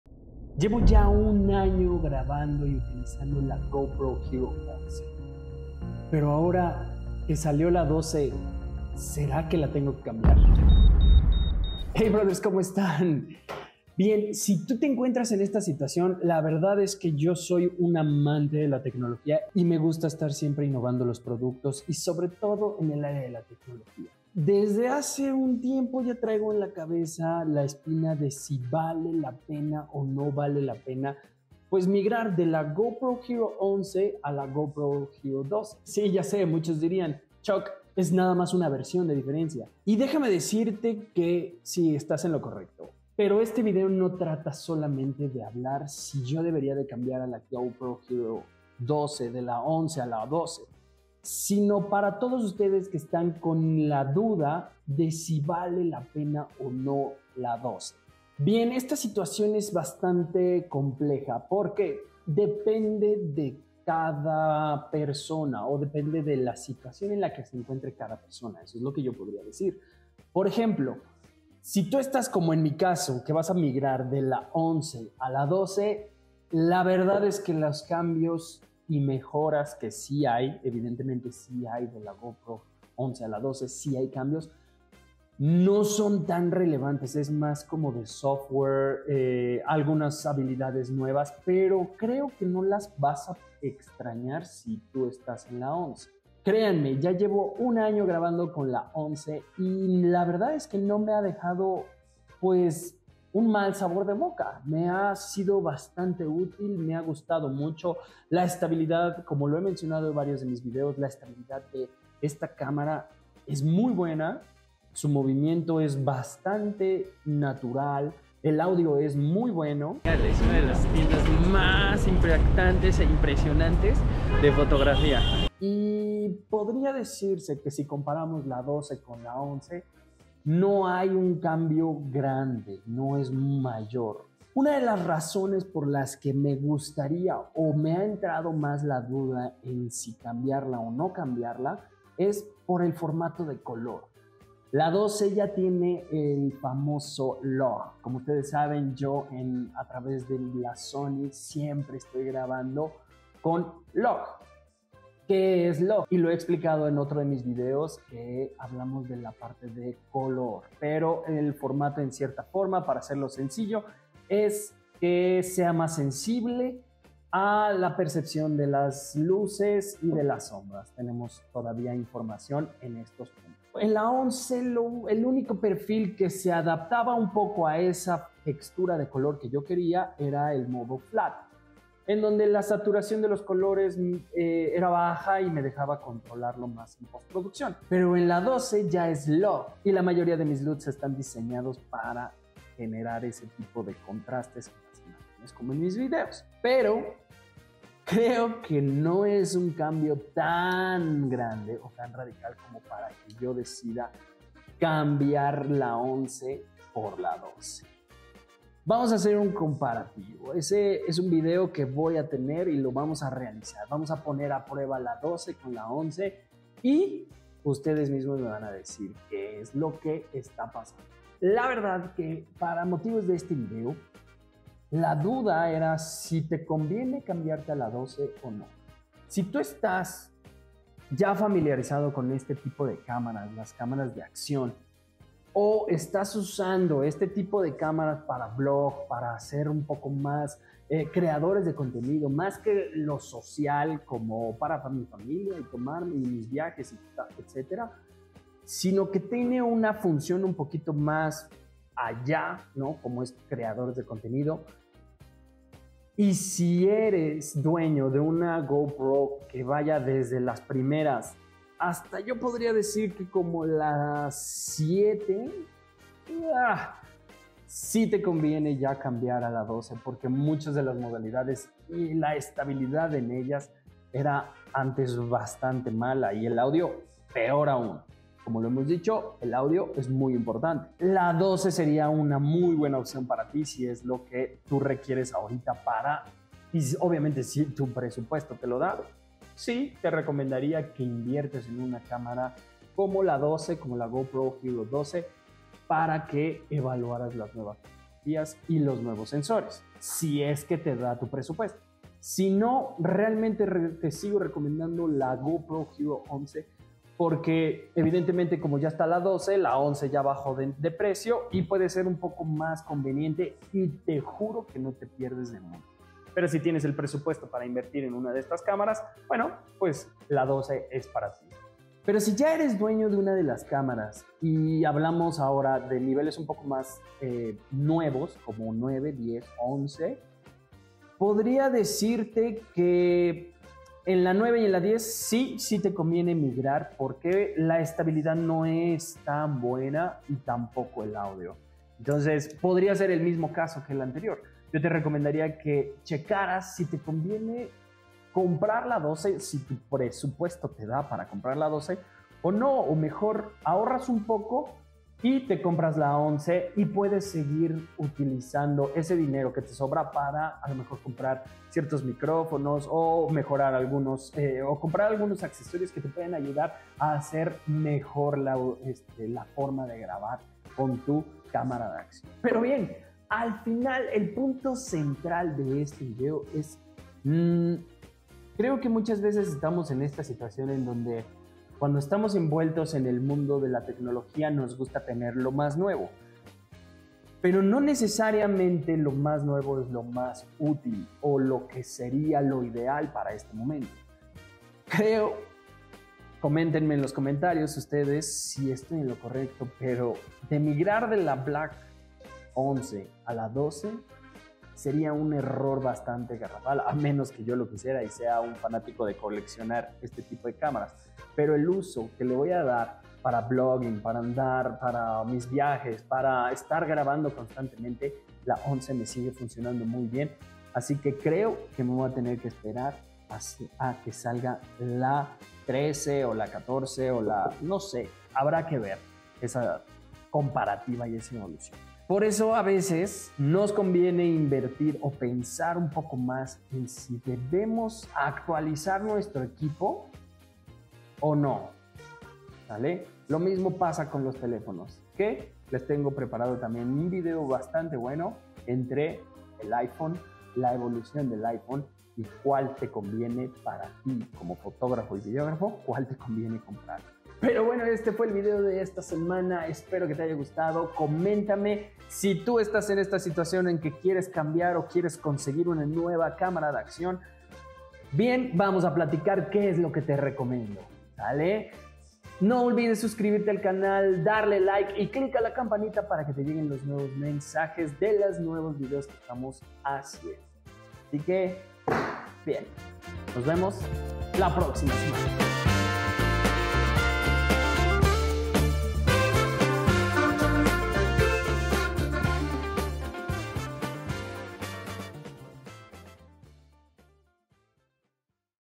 Lo que necesitas saber antes de comprar la Gopro 12 /
Ya tengo una GoPro, pero ya salió una nueva… mmmm debería cambiarla, seguro tiene mejores cosas y además es la nueva… pero estaré tirando mi dinero.

¿Estás pensando en comprar la nueva Gopro 12? Antes de hacerlo, asegúrate de mirar este video donde te contamos todo lo que necesitas saber sobre esta cámara de acción. Desde sus características hasta su precio, ¡te lo contamos todo!

Si eres un amante de los deportes extremos o simplemente quieres capturar tus viajes de una manera única, la Gopro 12 puede ser la opción perfecta para ti. Acompáñanos en este video y descubre si esta cámara es la indicada para tus necesidades y presupuesto. ¡No te lo pierdas!

GoPro hero 12 vale la pena?

Llevo 1 año usando la GoPro 11 en diferentes actividades y probando todas sus funciones y de verdad que no me arrepiento

Equipo
Camara: Sony a7iii